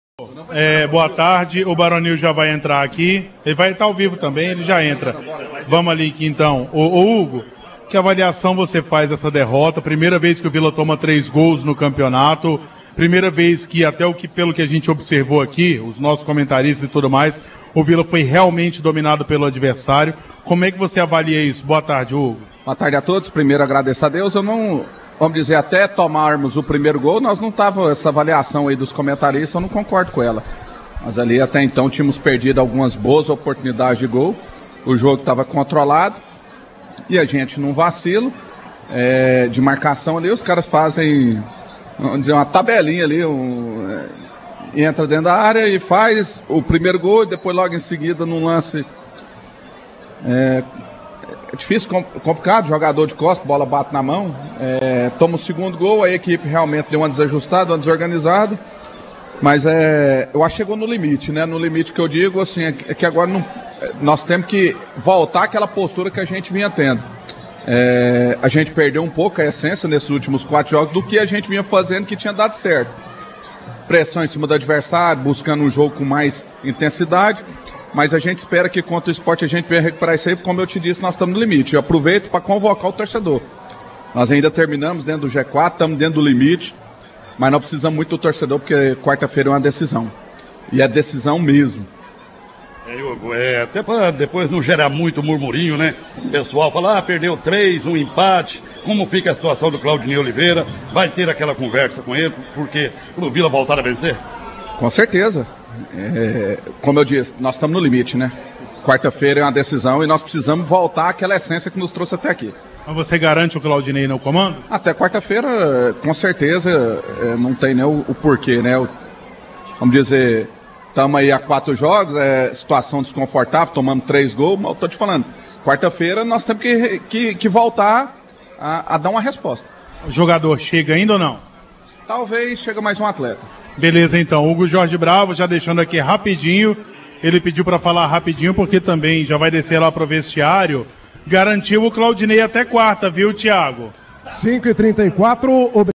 Hugo J Bravo fala que o Vila Nova está no limite e garante Claudinei até o próximo jogo
Neste domingo (30), pela série B, o Vila Nova enfrentou o Novorizontino fora de casa e infelizmente a vitória ficou com o Tigrão paulista, que saiu com um resultado de 3x1.